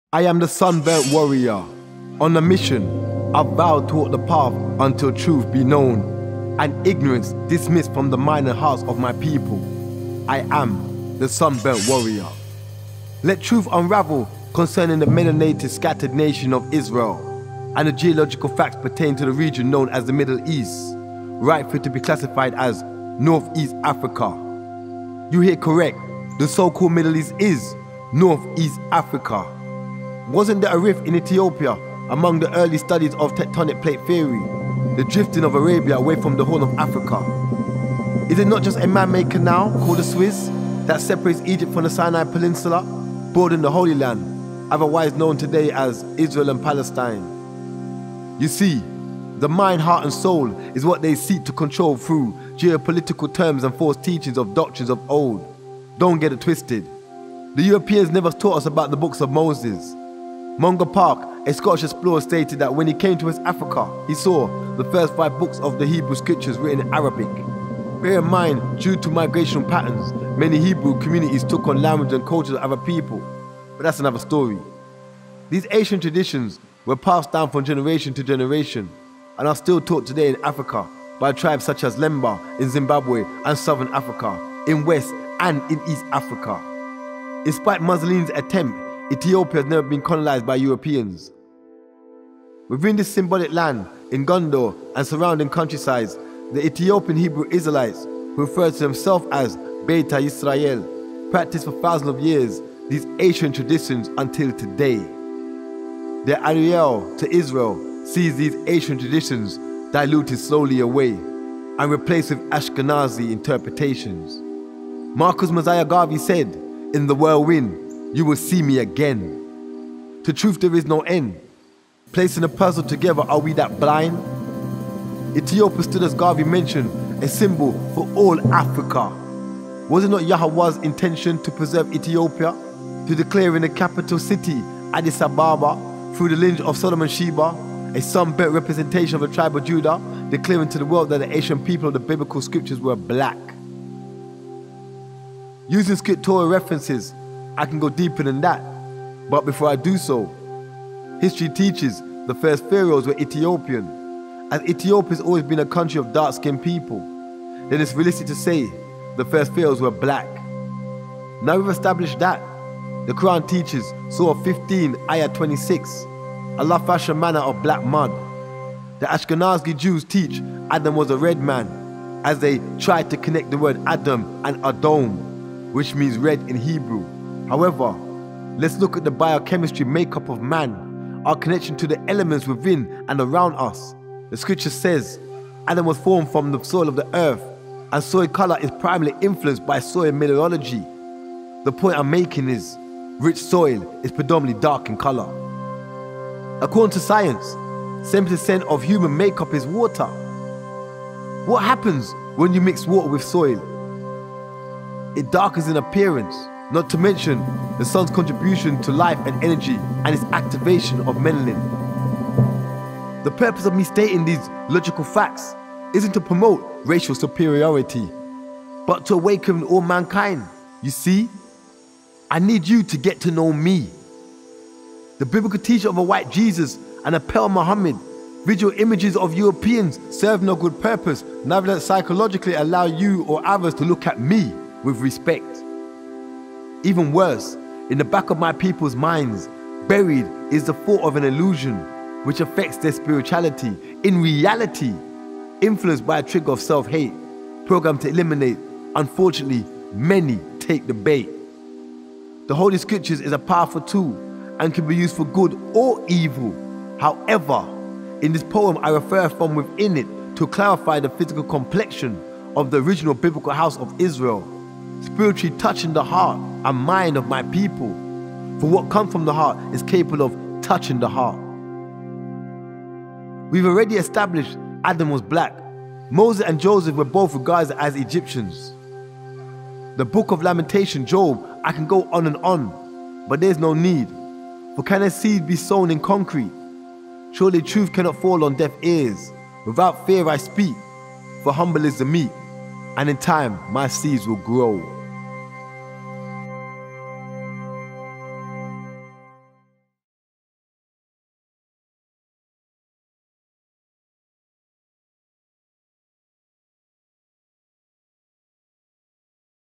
Sunburnt Israelite Warrior - Message to all Hebrews (Hebraic Poem)
Oneness the book coming soon. Exploring the Abrahamic books unveiling the foundations of the Children of Israel and their role.